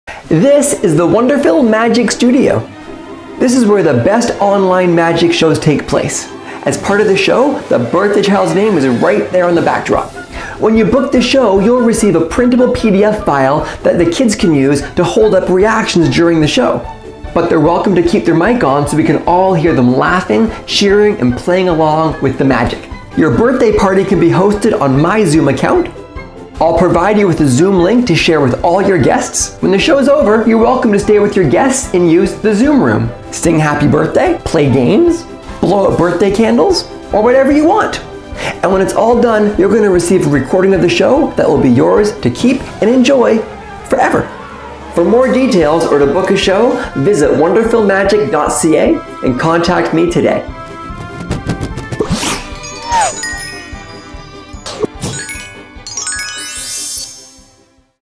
Hosting a Zoom Birthday Magic Show
Hosting a birthday party magic show on Zoom is easy!

Contact WonderPhil to book a show, and you're on your way to the best virtual birthday party around!